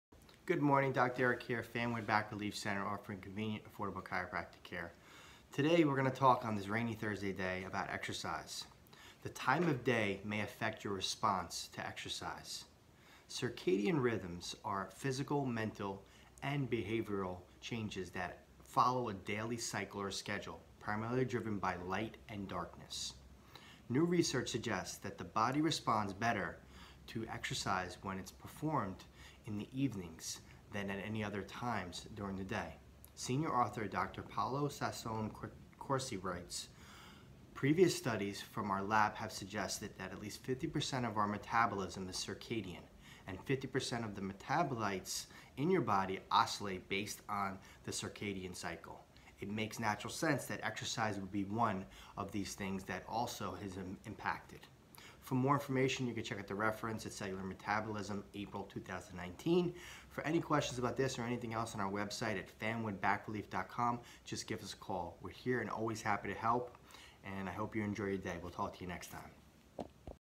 Fanwood & Scotch Plains Chiropractor Explains How Time of Day May Affect Your Response to Exercise
Fanwood & Scotch Plains Chiropractor Eric Chludzinski of Fanwood Back Relief Center explains how time of day may affect your response to exercise.

Back pain.
Neck pain.
Accident injury and whiplash.
Headaches.
Sciatica.

By:
Fanwood Back Relief Center